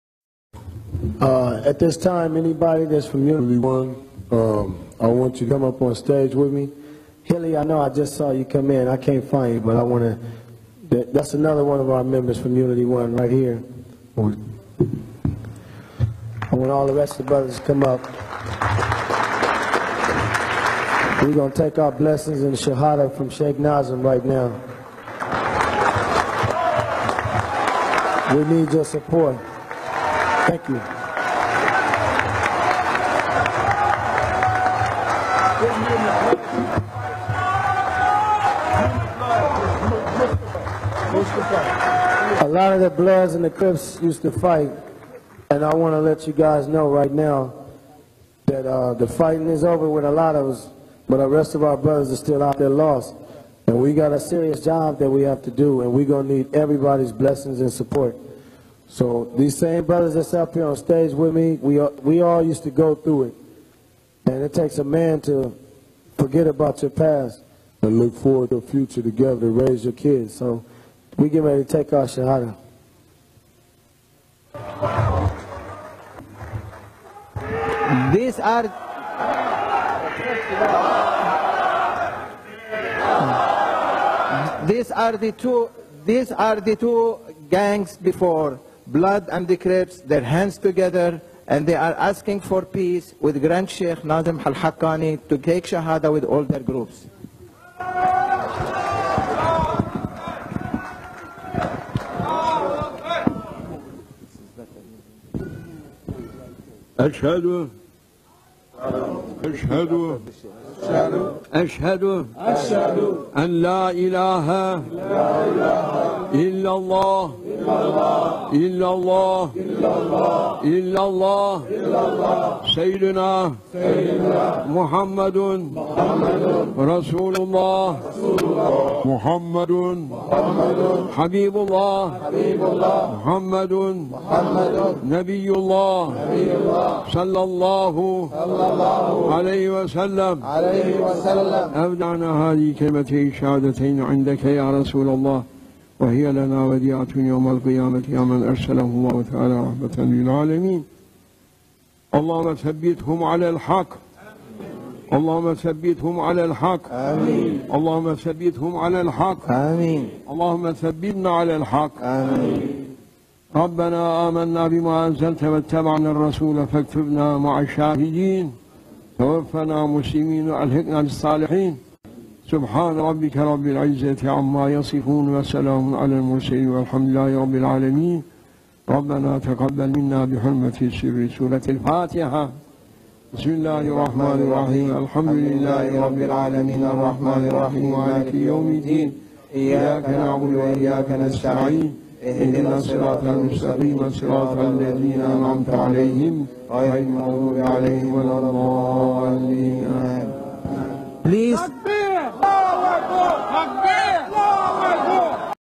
Mawlana Sultan Awliya Shaykh Nazim (Qs) 1996 CRIPs & Blood gangs took Shahadah & Bayah
In 1996 in Los Angeles Mawlana Sultan Awliya Shaykh Nazim (Qs) made peace between the CRIPs and Blood gangs in Los Angeles, These 2 gangs were at war with each other for years and took Shahadah & Bayah